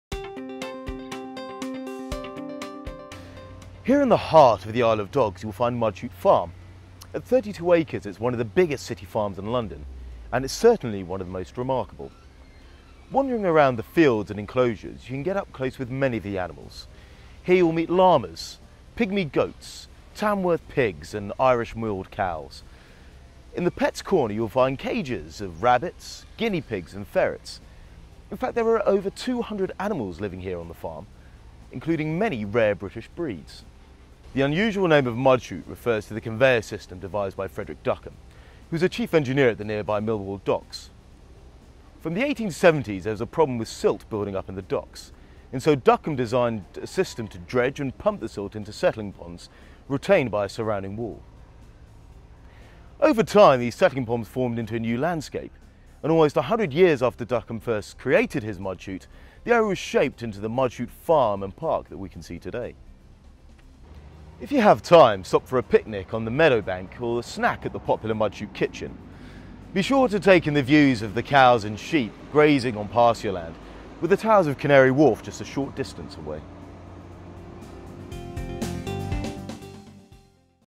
A Riverside Meander - Mudchute
Discover Tower Hamlets Borough through a number of entertaining and inspiring walks which submerge users in the past and present of this colourful and diverse area of East London.